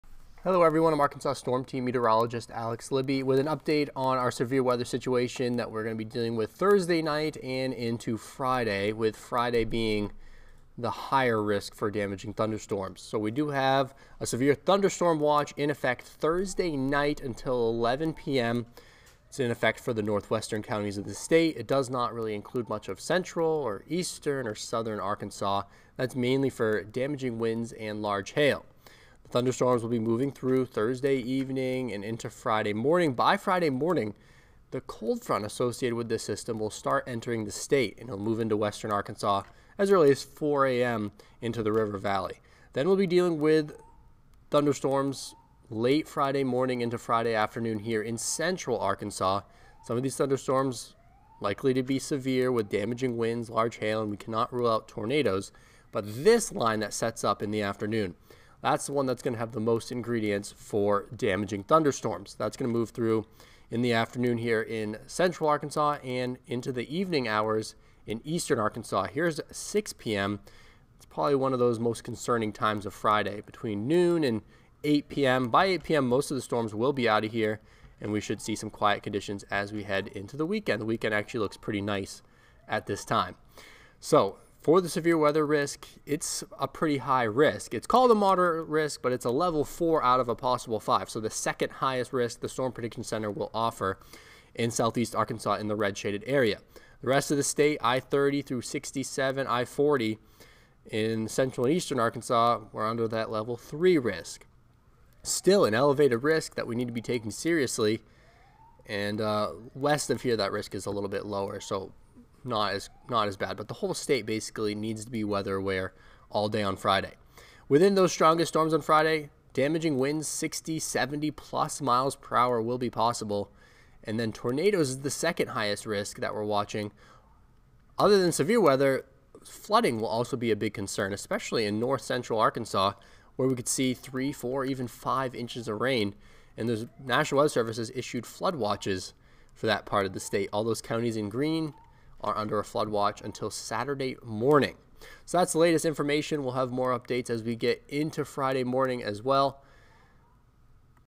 Tornadoes, damaging wind, and flooding to impact Arkansas Friday 2/24/2023
Arkansas Storm Team Meteorologist Alex Libby explains the timing and impacts of this substantial severe weather threat.